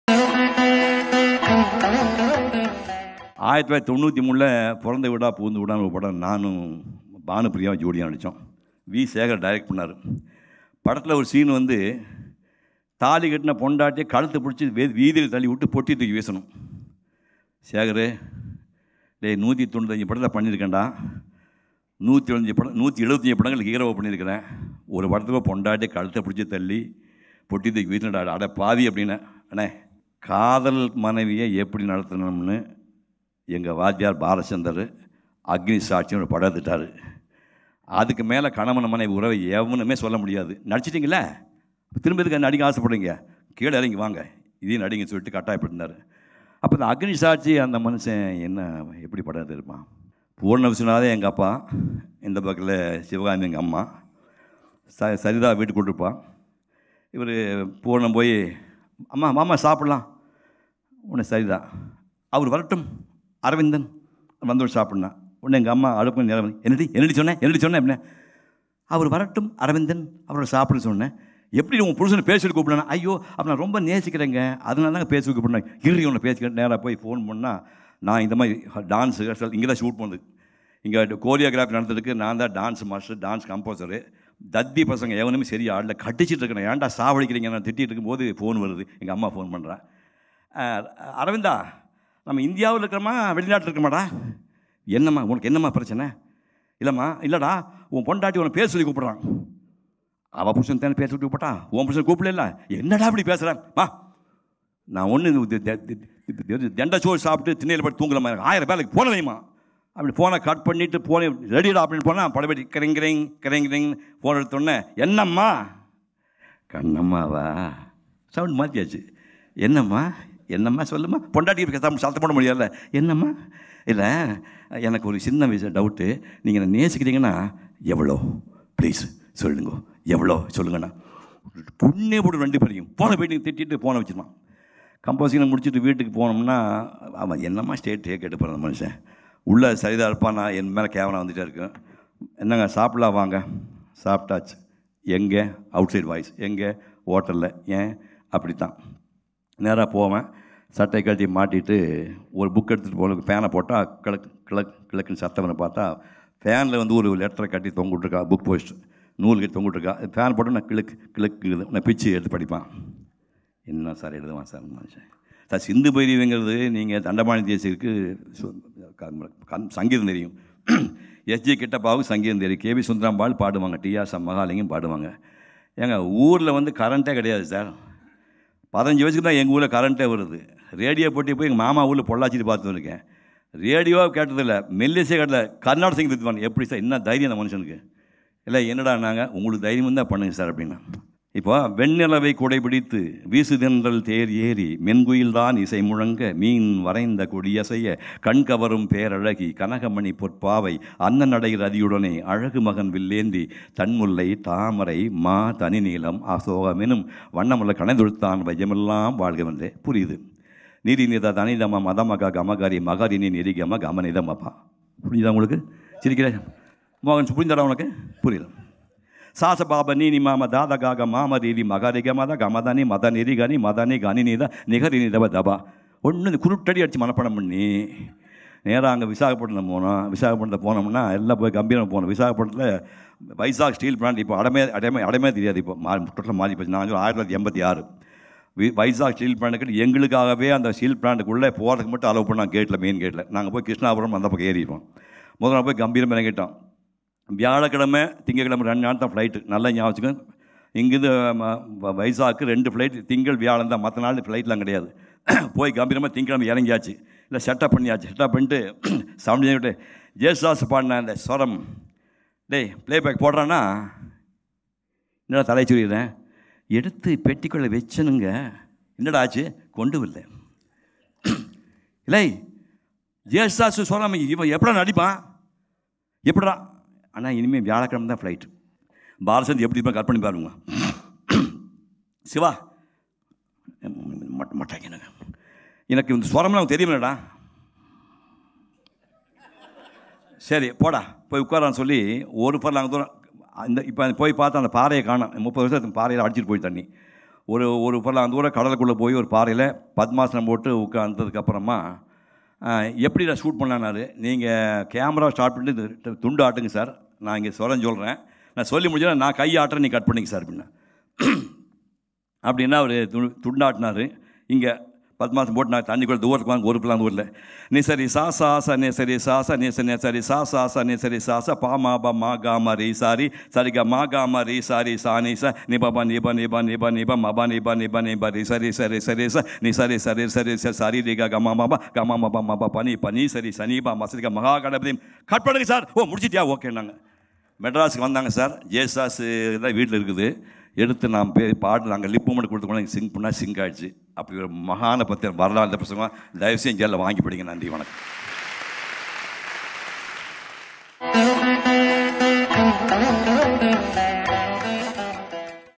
சிந்து பைரவி - கர்நாடக இசைக்கலைஞராக நடித்தது எப்படி? மனம் திறக்கும் சிவகுமார்
கர்நாடக இசைக்கலைஞராக சிந்துபைரவி படத்தில் நடித்தது எப்படி? மனம் திறக்கும் சிவகுமார்...

மேலும், பார்க்க...